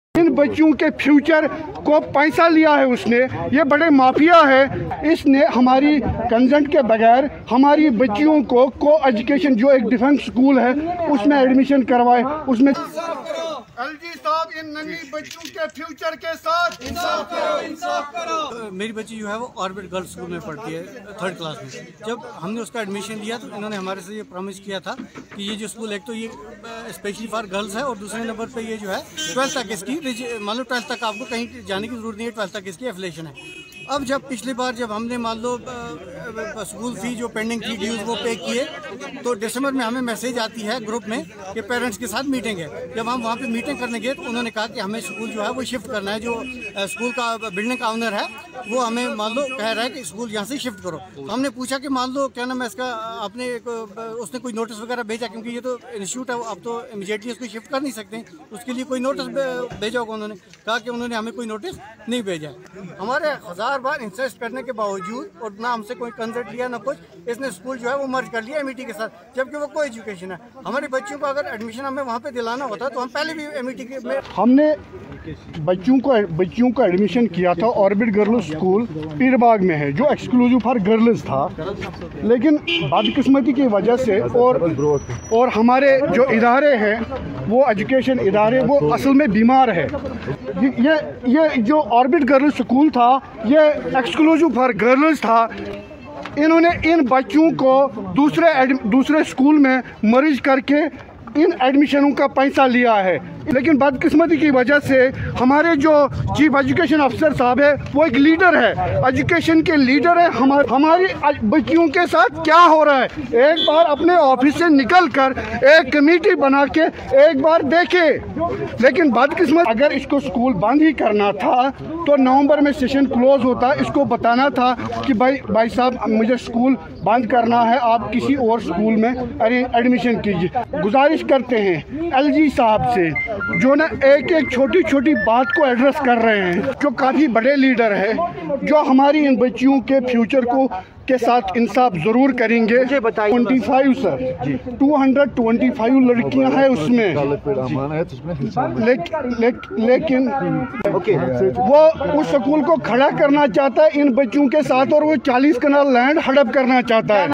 Protest against merging of a private school in Srinagar
Parents and students Monday staged a protest against the merging of a private school against their will in Srinagar.

The school in question is ‘Orbit Girls School’ located in Peerbagh Srinagar that according to parents has been merged with ‘World Foundation School’ without taking them into confidence.

The protesters raised slogans against the school administration and concerned authorities.

“We received a WhatsApp message from the Principal of the ‘Orbit Girls School’ on 17 December. We thought parents have been called over ‘fee row’ but to our shock, we were informed that this school has been merged with another school. The school administration took this unilateral decision after collecting fee from all students,” one of the protesters told news agency Kashmir News Trust.

“‘We are against this merging and seek the intervention of LG Manoj Sinha and Director Education. We have been deceived,” the protesters said.